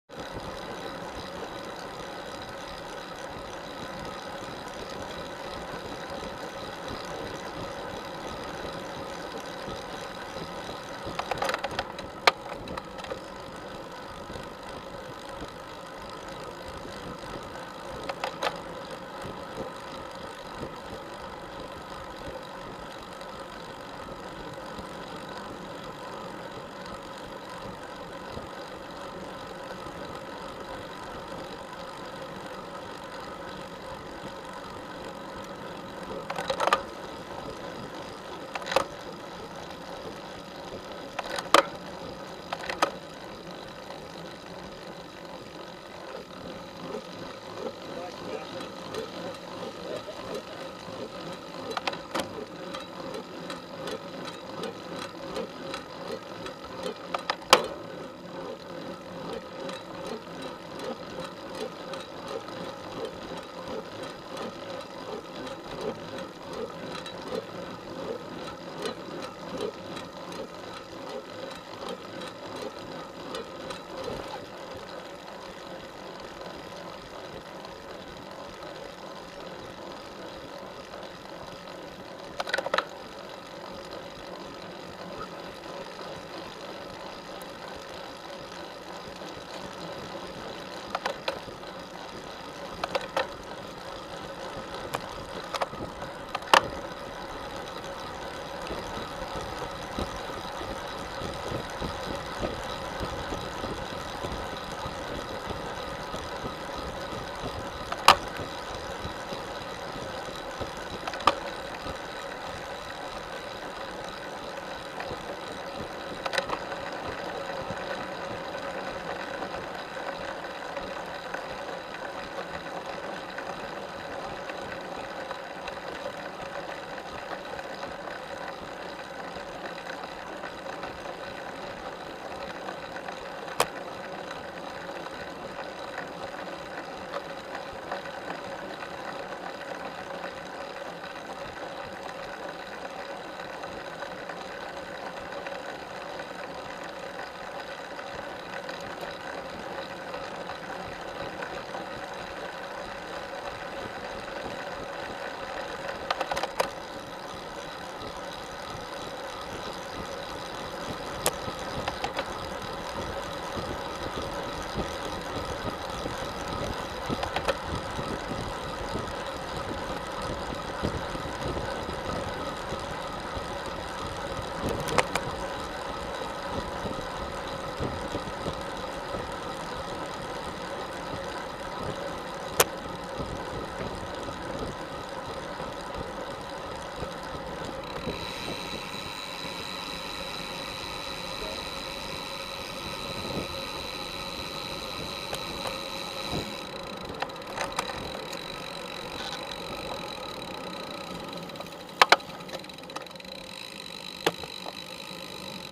Million dollar hill
Million dollar hill, Alpharetta LTF sunday morning ride.
Someone did not get the memo, this is MY hill! Nobody's supposed to catch me on it!